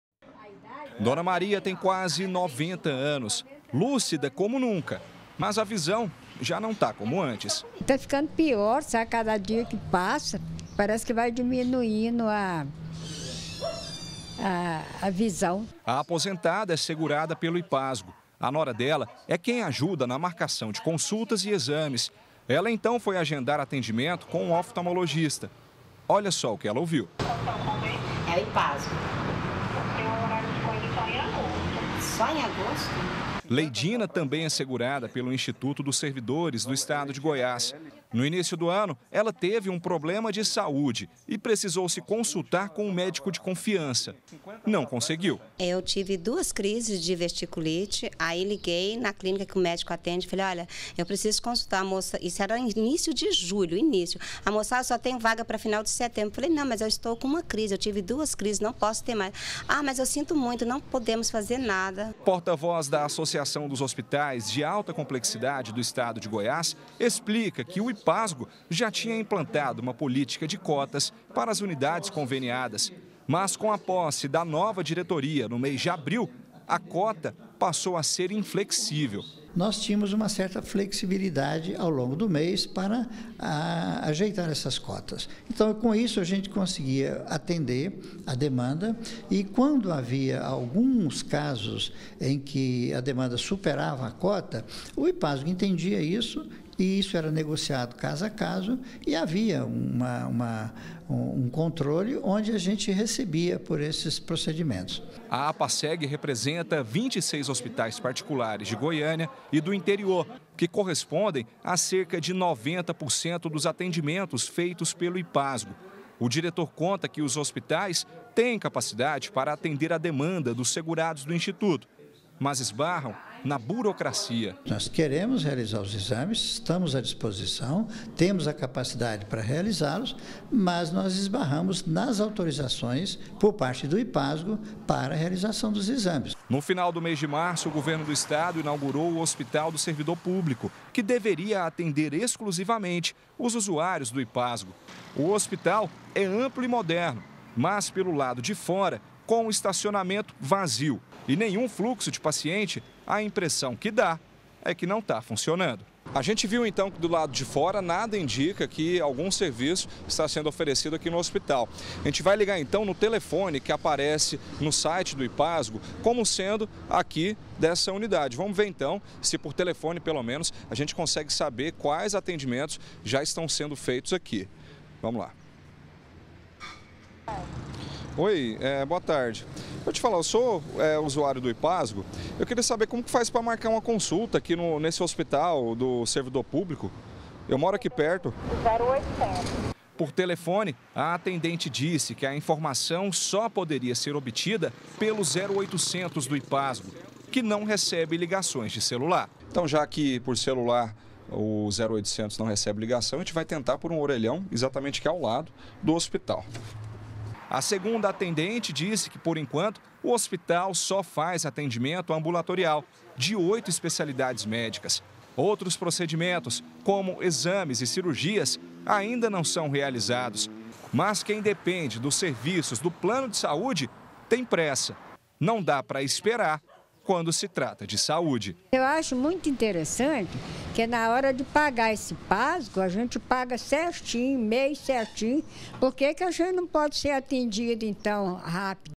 GR - IPASGO: Clientes de plano de saúde não conseguem consulta - 16-05-2018
O IPASGO é o maior plano de saúde do estado. São quase seiscentos e cinquenta mil usuários. O problema é que de uns tempos pra cá os segurados reclamam que tá a maior dificuldade marcar uma consulta ou exame.